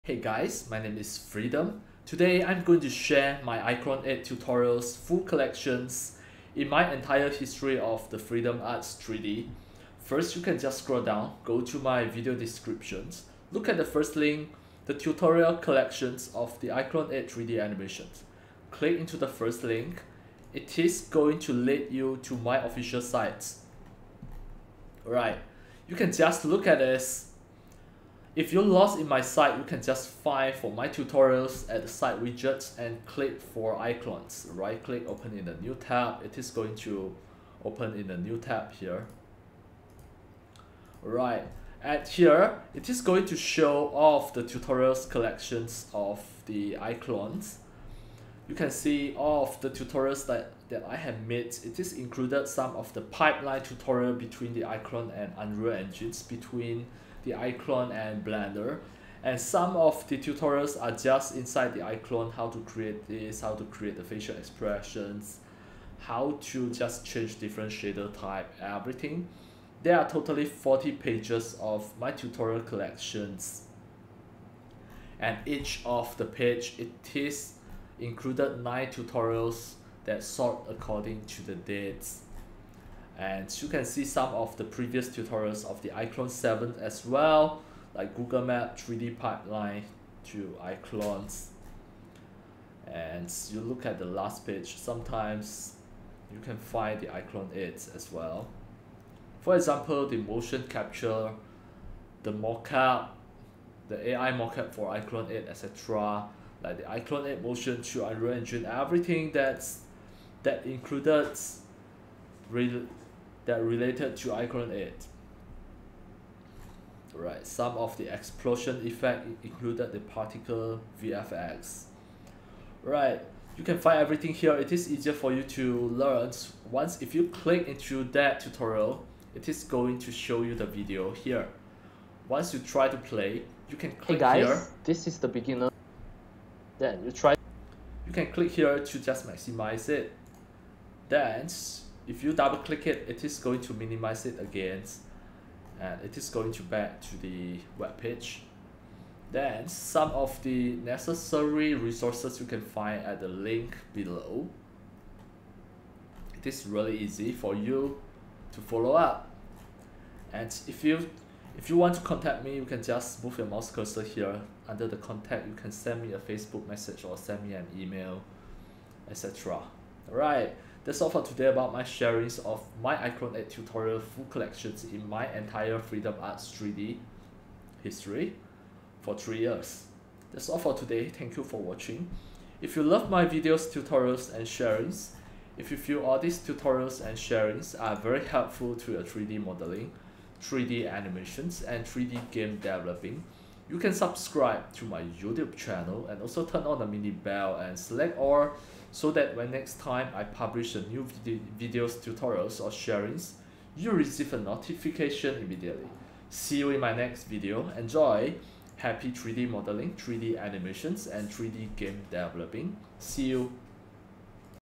iClone 8 Tutorial Collection - 3D Animation Pipeline - Freedom Arts 3D
Description:
hey guys, this is my iClone 8 tutorials collection which included all my iclone 3D animation, game dev pipeline, facial mocap, body mocap tutorials etc! It is a massive tutorial collection which I categorized it systematically, easier for you to find! ENJOY! and having fun! Happy 3D Modeling, 3D Animation + 3D Game Dev! ENJOY!

iClone,
Tutorial,
Freedom Arts,
Collection,
Reallusion,
Character Creator,
CGI,
3D Modeling,
3D Animation,
Game Dev,
Tutorial,